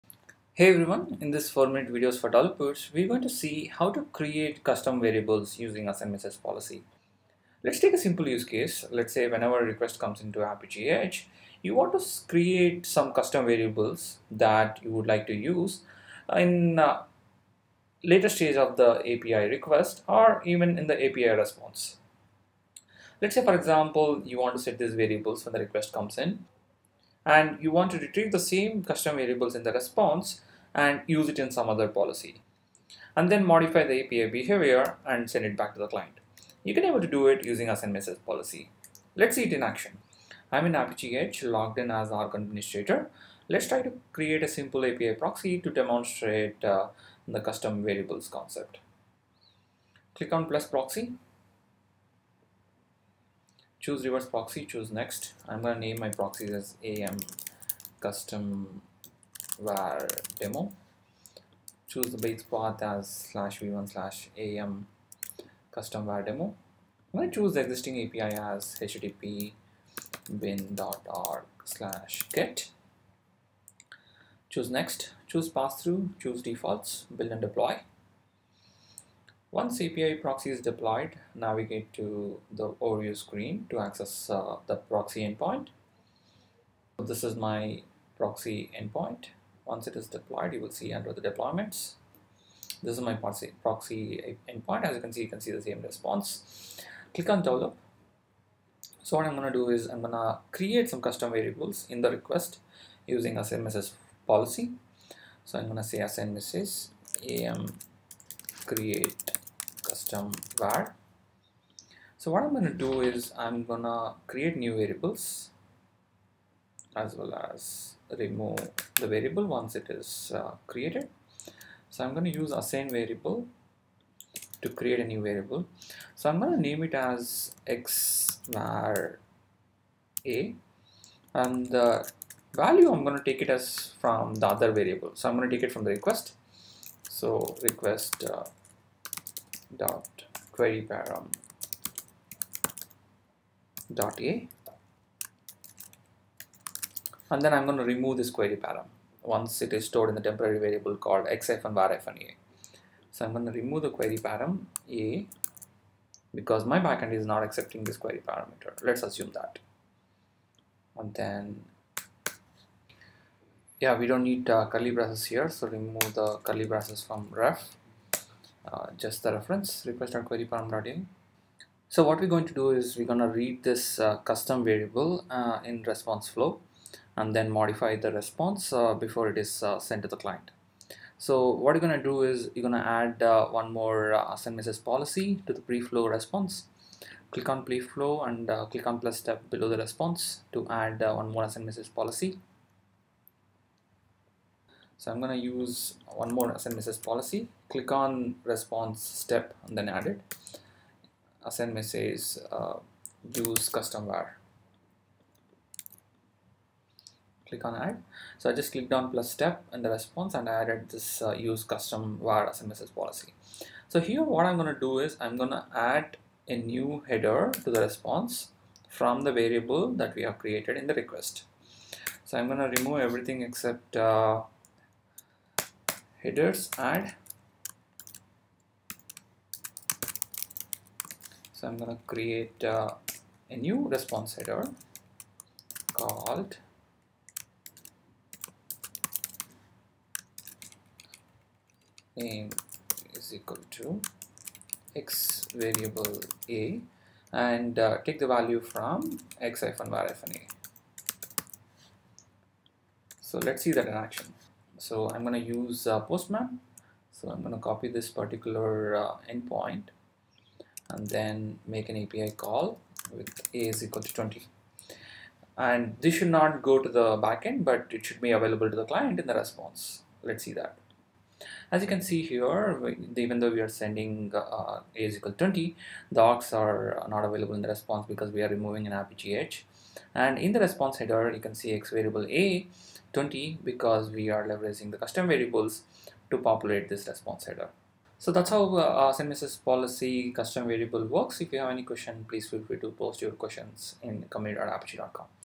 Apigee Edge - 4MV4D - Assign Message Policy- Create Custom Variables - S09E05
In this 4MV4D, Find out how to set custom flow variables in Apigee Edge using Assign Message Policy & Leverage same in other policies in API Proxy.